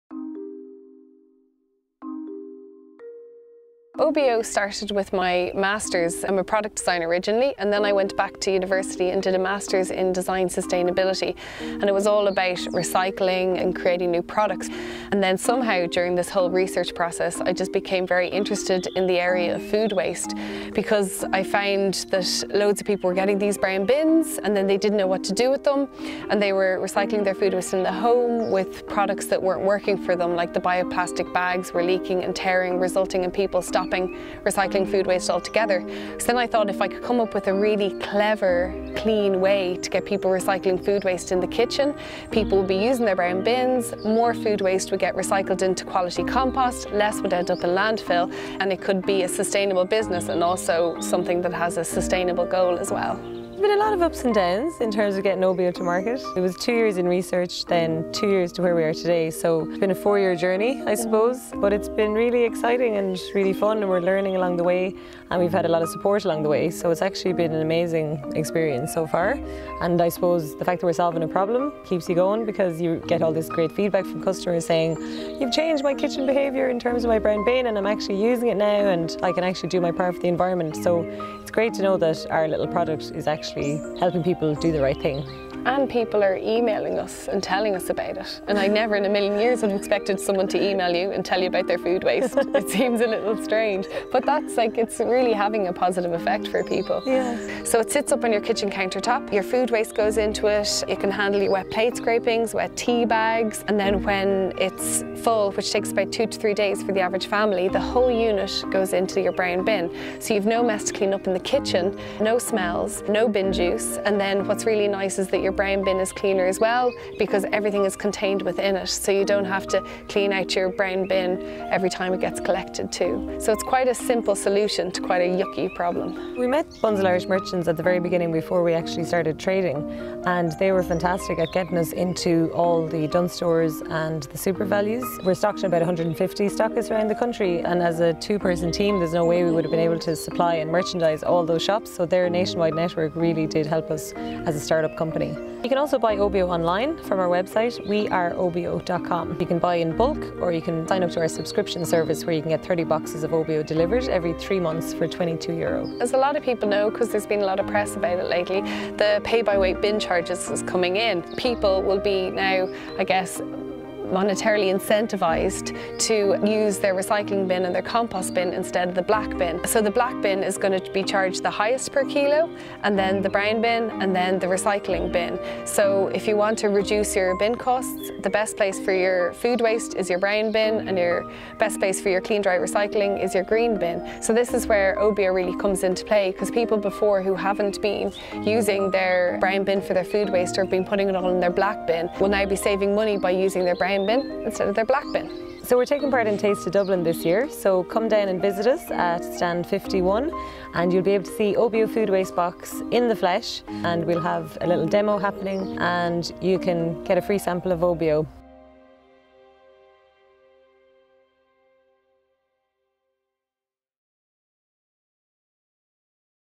Obeo
For our final Taste of Dublin exhibitor video we talk with Kate and Liz from Obeo
Kate developed the idea for a fully compostable kitchen counter-top alternative for food waste while at college, it was there that she teamed up with Liz to make the idea a reality.
From our own perspective, Bunzl Irish Merchants Coffee Shop is proud to be involved in a very small way as their distributor over the last few years, and, following their strong showing on Dragons' Den - Ireland we're sure they'll continue to go from strength to strength in the coming years.